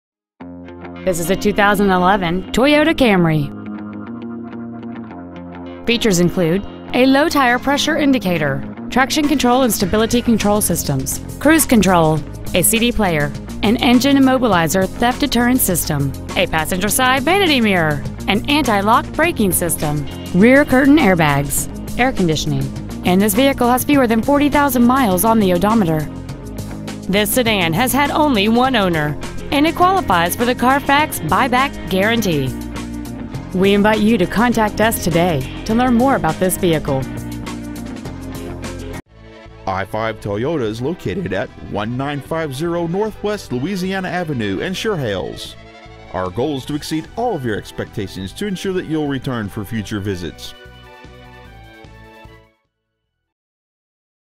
2011 Toyota Camry Olympia WA
Year : 2011
 Make : Toyota
 Model : Camry
 Engine : 2.5L 4Cyl

 Exterior : Green
 Miles : 39,297

 2011 Toyota Camry Chehalis WA